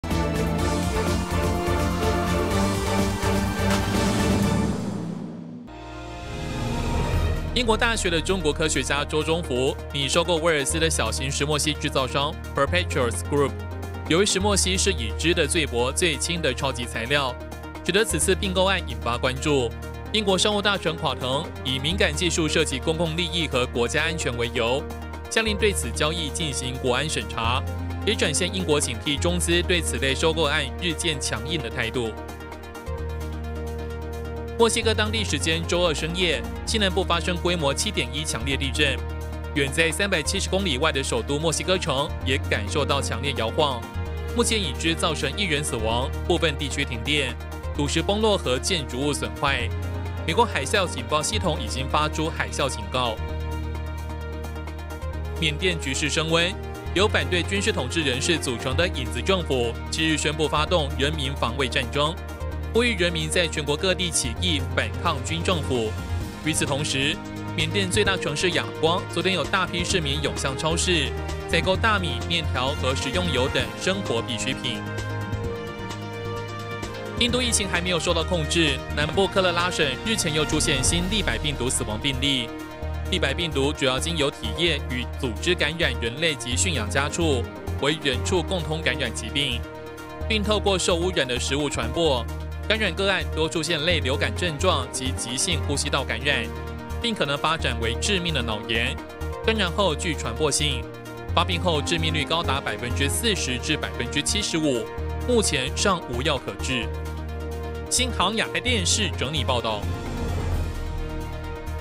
中資擬收購奈米關鍵材料石墨烯公司 英介入調查｜寰宇掃描
🔸中資擬收購奈米關鍵材料石墨烯公司 英介入調查
🔸規模7.1強震撼墨西哥 發海嘯警報至少1死
🔸緬甸「影子政府」向軍政府宣戰 仰光現搶購潮
🔸立百病毒重現印度南部 地方政府警戒